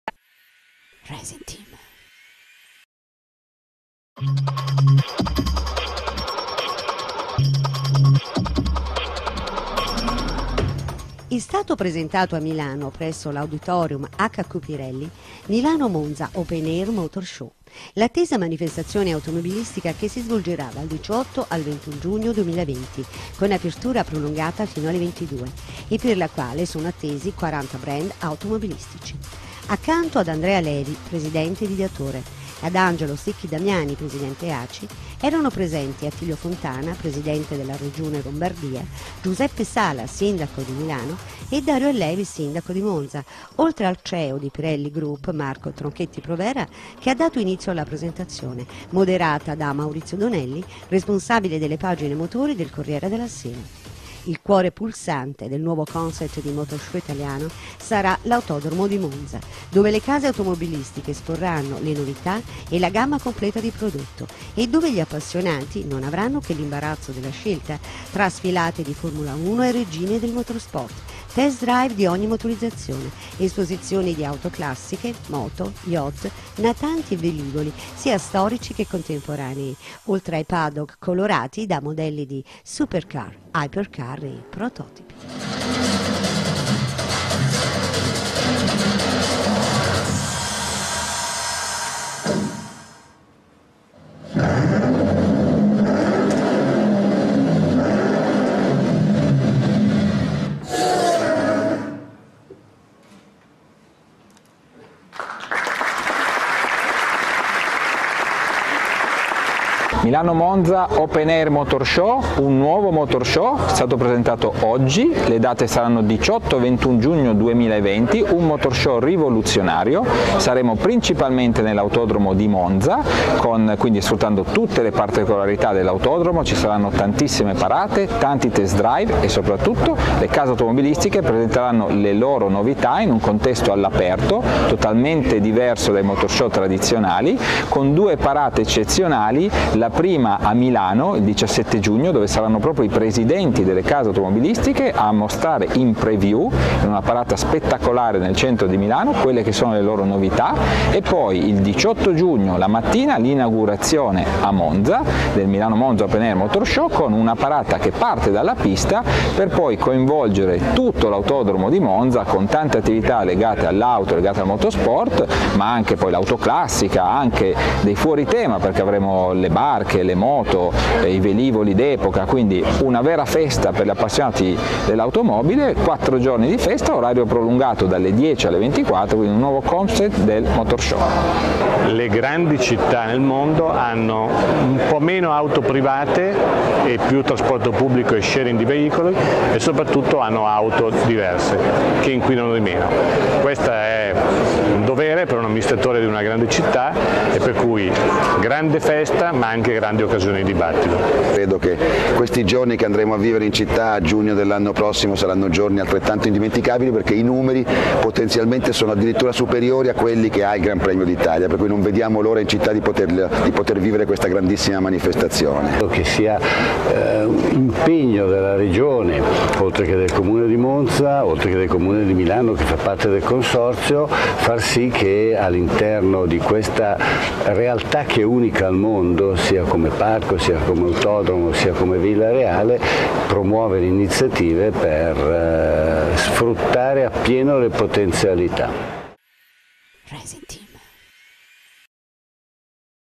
Presentazione di "Milano Monza Open-Air Motor Show"
La nuova manifestazione automobilistica si svolgerà dal 18 al 21 giugno 2020